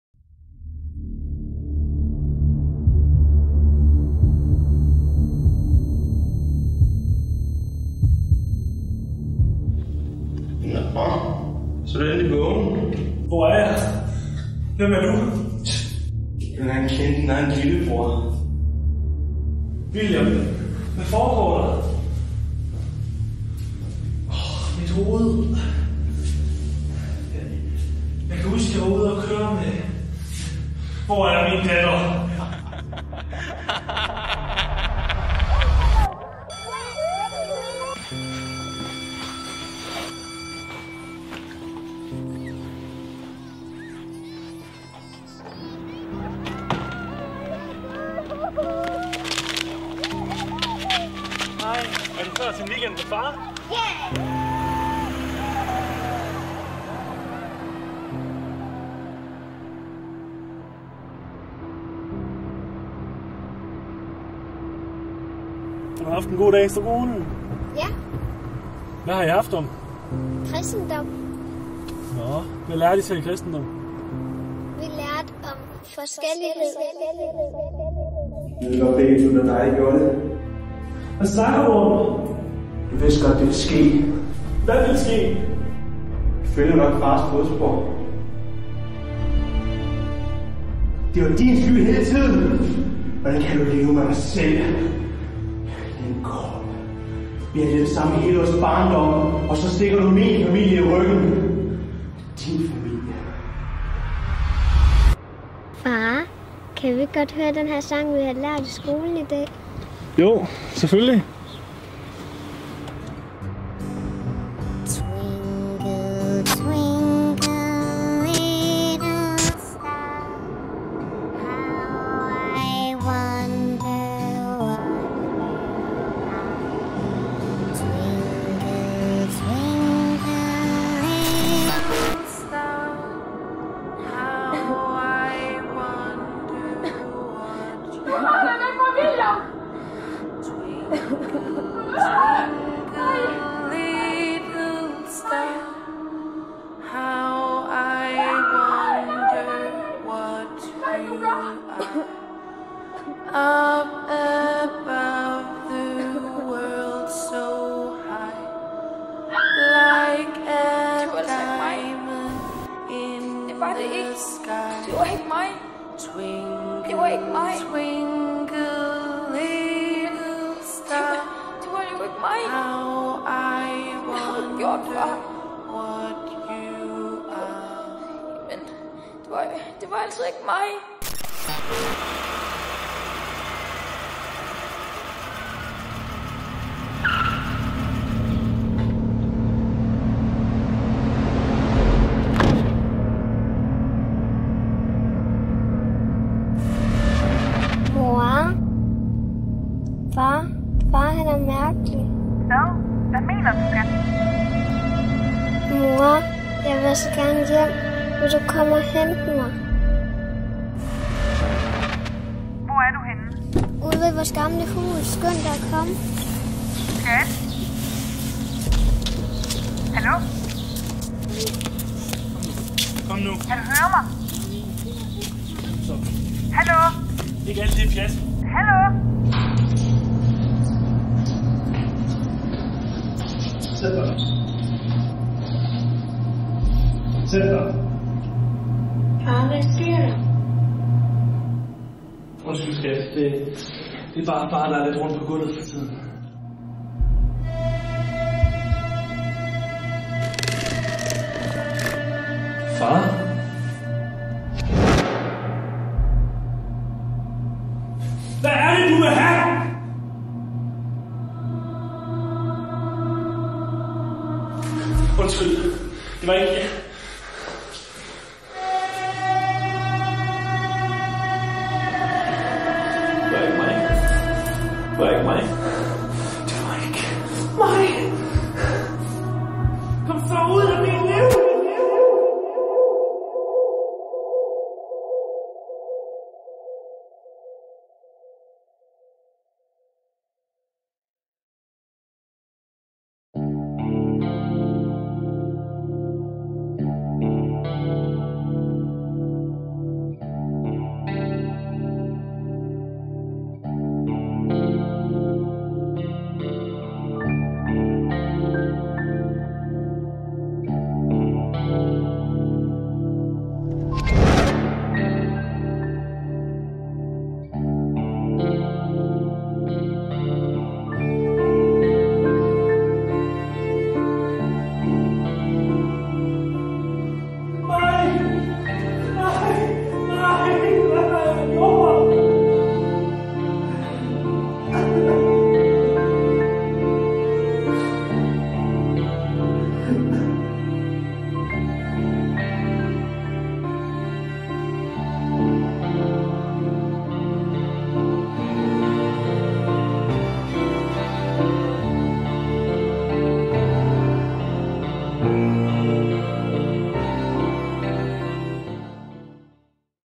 Det var ikke mig
En mediefag C eksamensfilm fra VHG Vesthimmerlands gymnasium og HF af Andreas, Morten og Emilie

Filmen deltager i TheNextFilmFestival 2017
Kategori: U21
Region: Nordjylland
Instruktør: Andreas Furbo Jørgensen